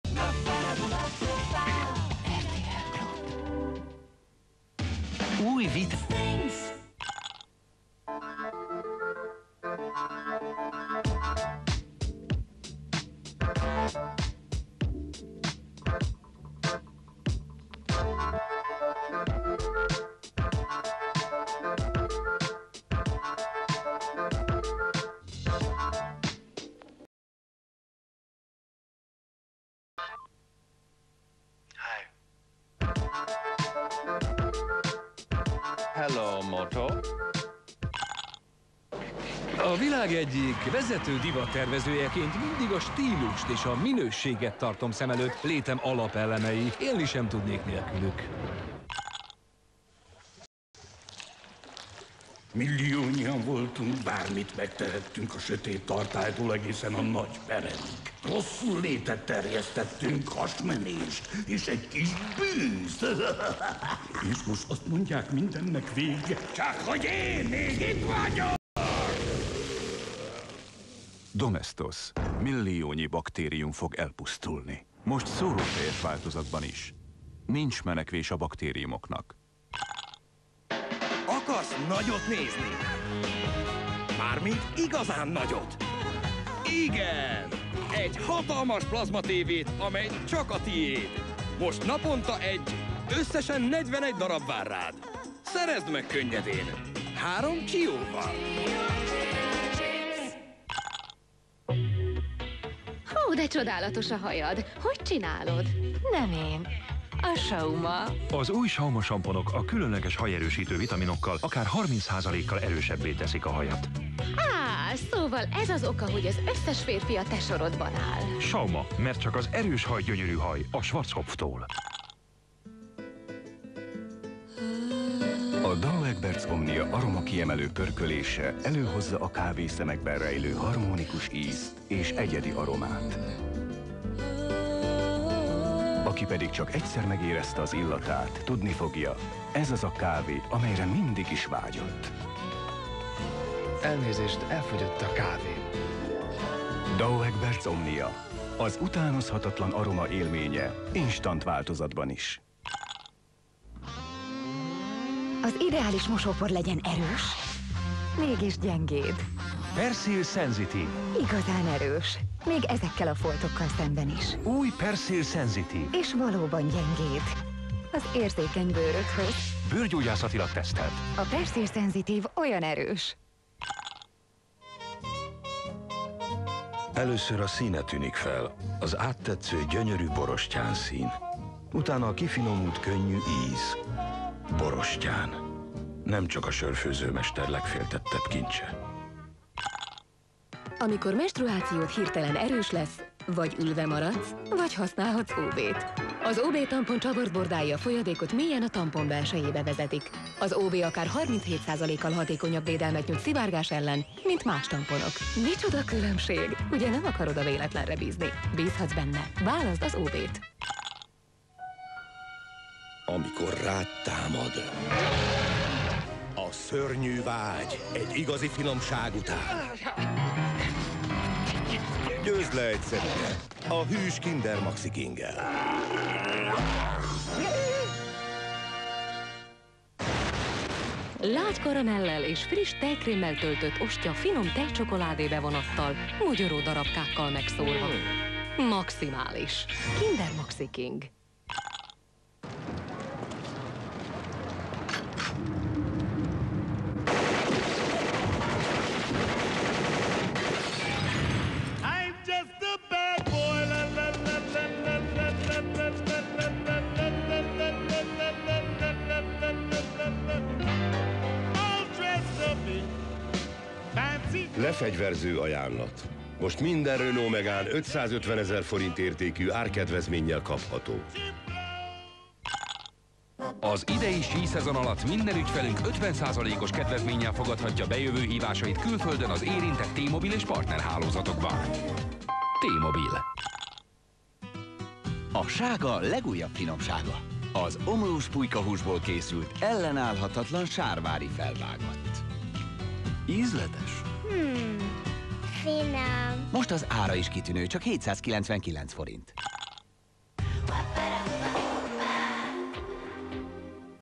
RTL 2005 ősz 2. rész
A felvétel 2005 őszén készült reklámokat tartalmaz, kis nosztalgia.